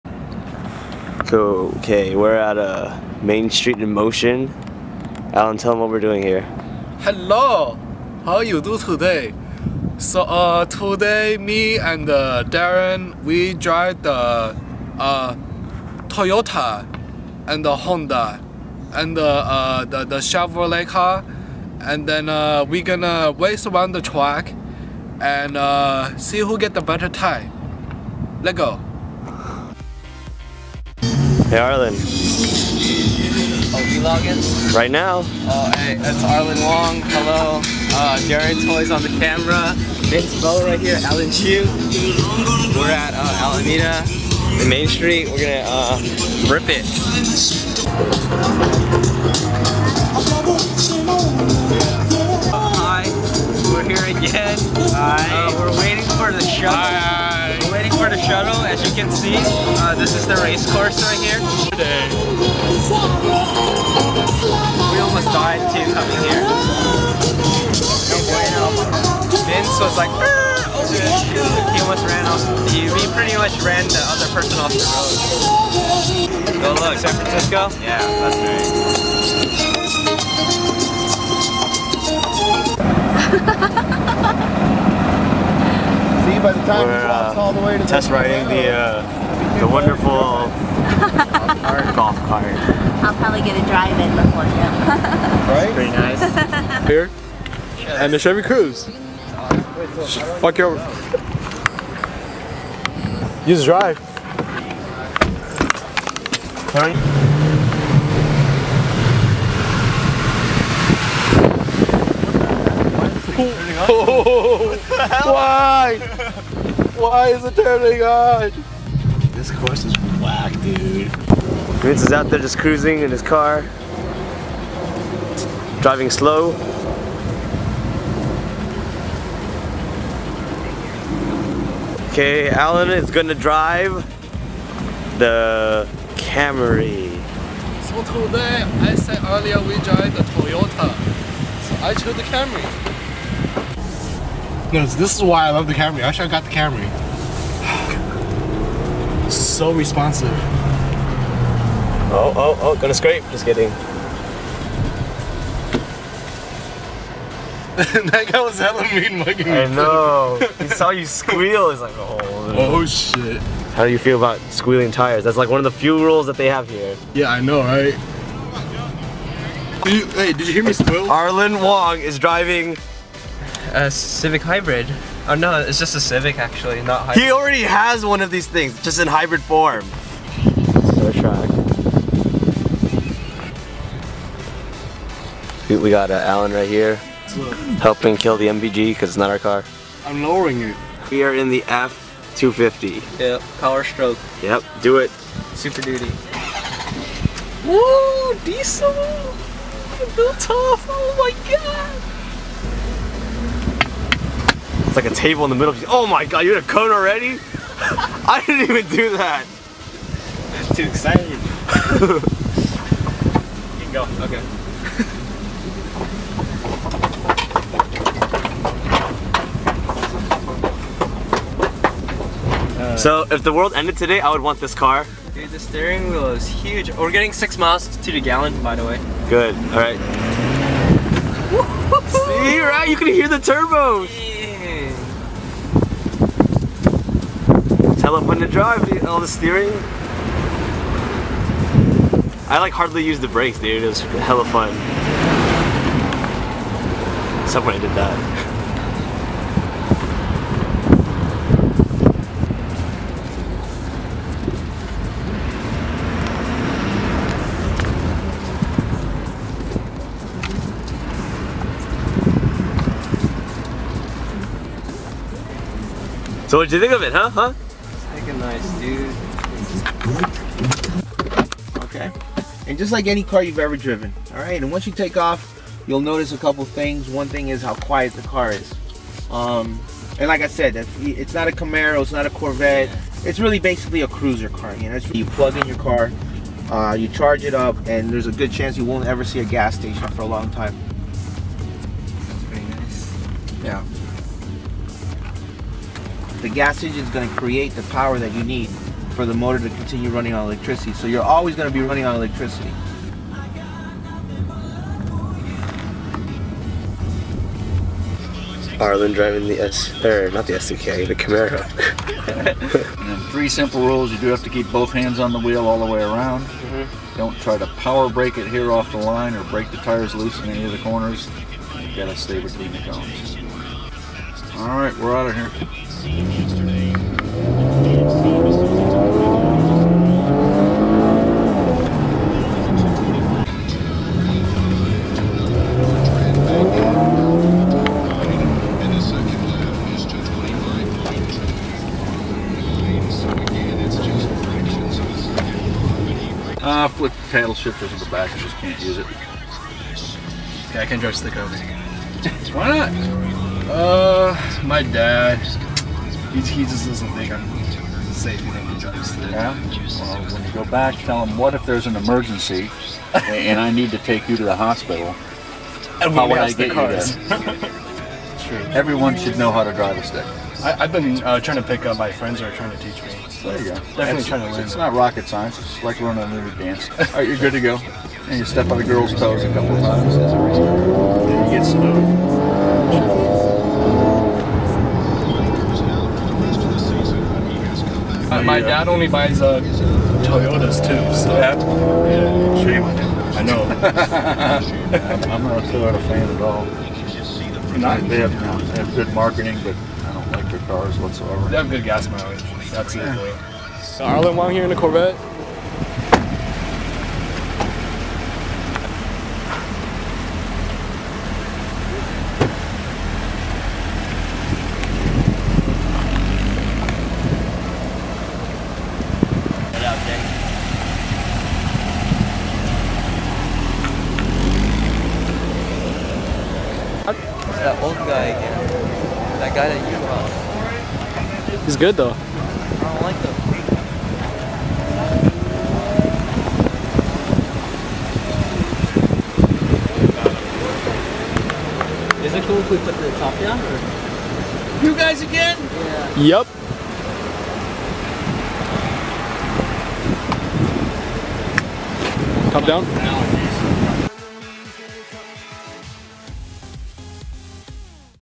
MainStreet in Motion 2011 Vlogg
arlin alan vince darren go out to main street in motion in alameda to test drive some cars

sadly this event has been discontinued but may start up again. these clips were from almost a year ago but i uploaded and put them together now...more because i found them sitting in my external hard drive